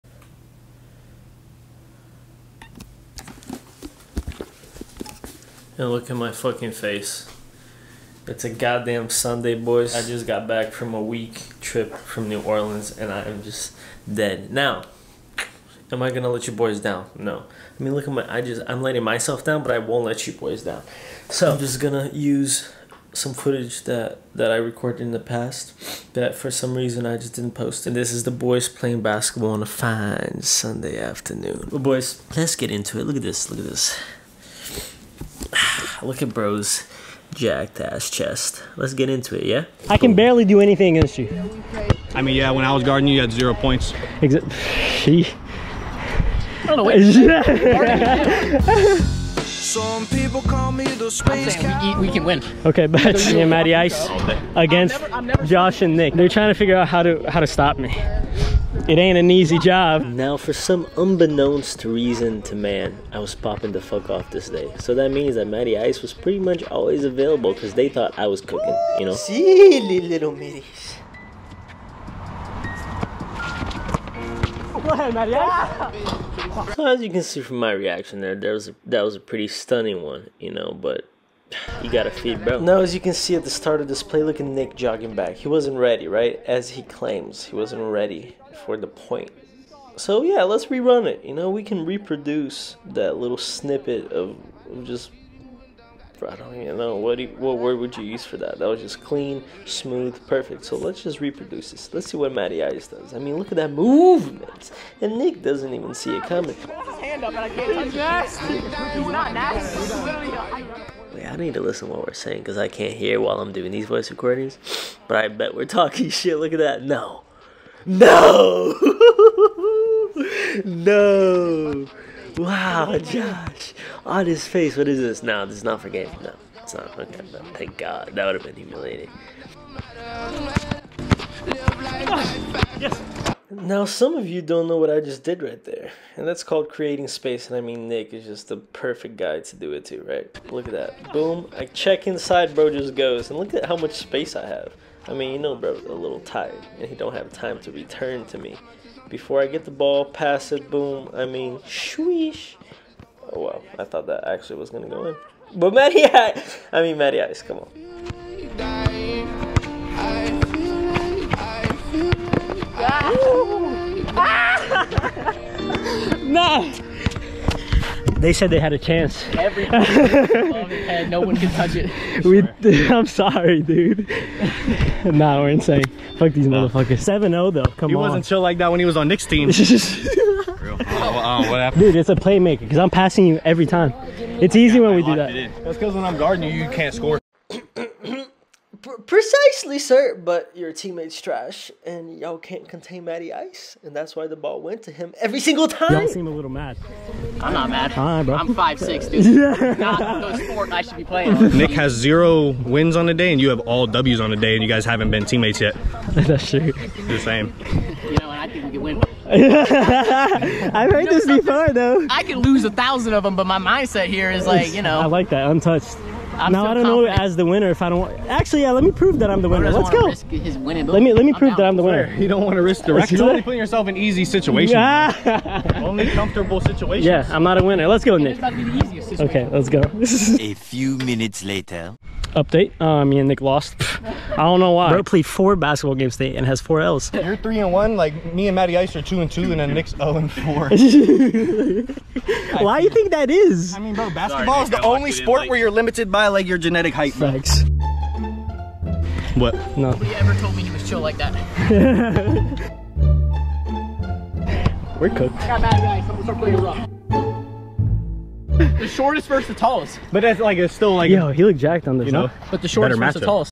This Is What Happens On A Sunday Vlog...
Welcome to vlog 384!!! In this episode the boys play a crazy game of basketball.

Timecodes:
00:00 - Intro
00:51 - Game
02:56 - Another kinda Game
04:07 - Outro